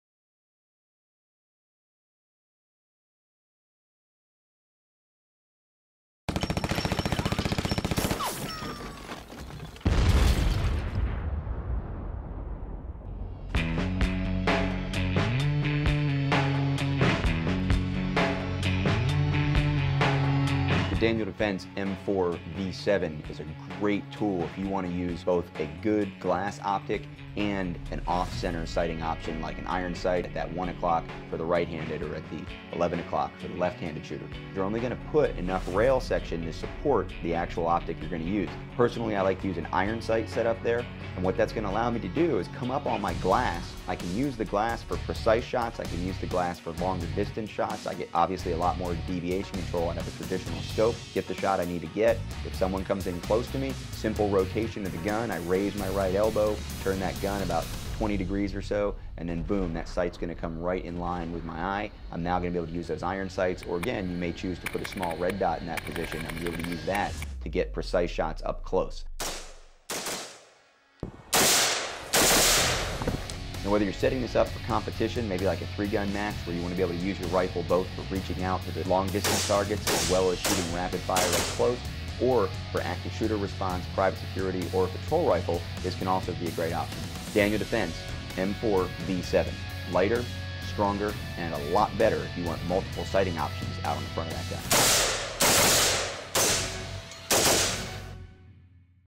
2012 Daniel Defense Video Series: Multiple Sighting Options (M4v7)
Rob Pincus discusses placing a set of iron sights on a rifle next to a traditional glass scope. This can be a great set up for an Active Shooter Response rifle, competition gun, patrol rifle or even for hunting. The Daniel Defense M4v7 can be set up perfectly for any of these applications.